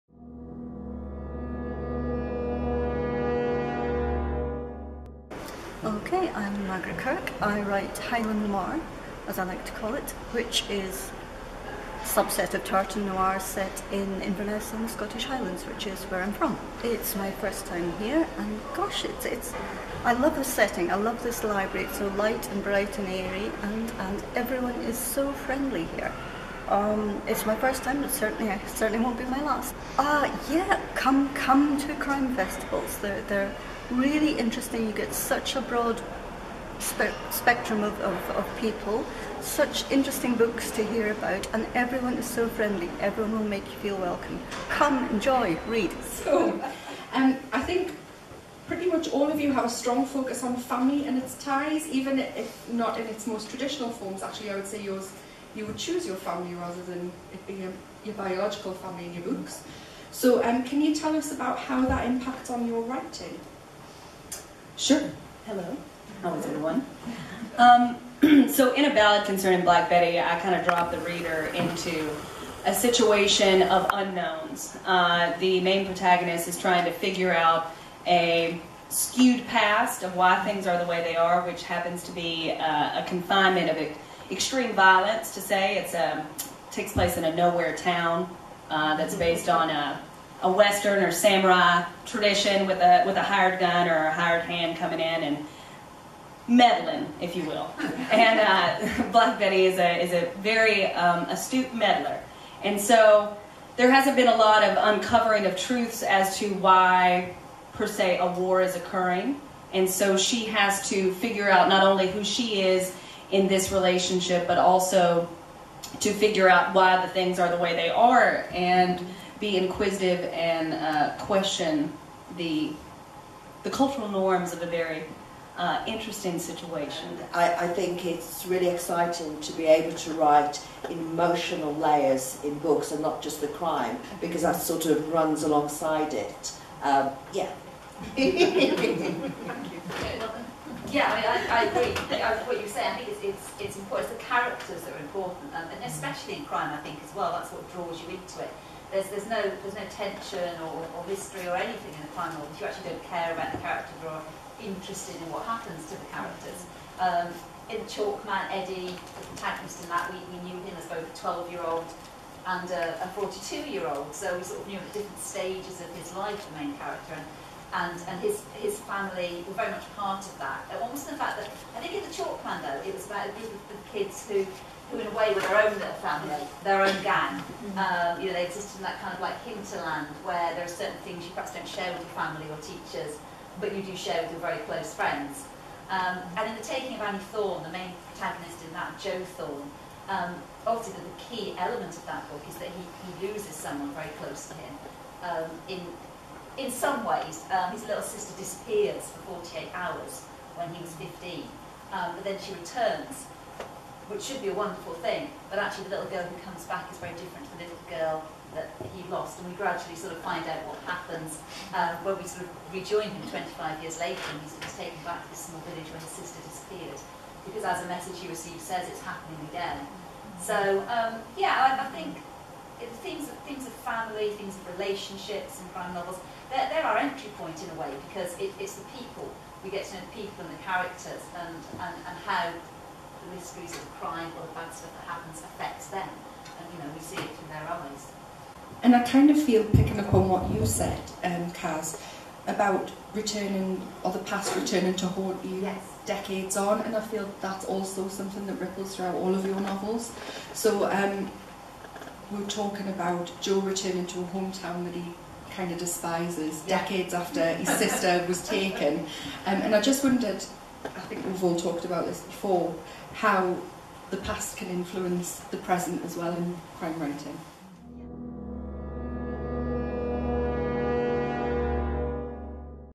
NN2019 - Criminal Magic
Deadlier than the Male
Jacqui Rose, CJ Tudor & Margaret Kirk

It will hardly surprise Noir audiences if we claim that it's perhaps the female imagination that carries us to the darkest deadliest shores. If you needed proof, here it is.